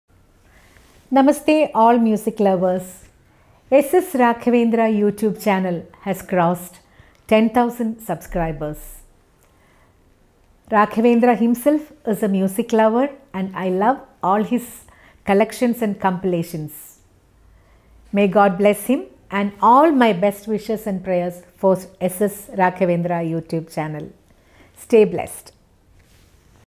Wishes from Padma Bhushan K S Chithra Amma
I am extremely happy and feeling blessed.  Wishes from  Padma Bhushan Dr.K.S.Chithramma for ordinary fan.  Getting blessings from Goddess Saraswathi is unforgettable moment.  Thank you Chithramma, Love you Always.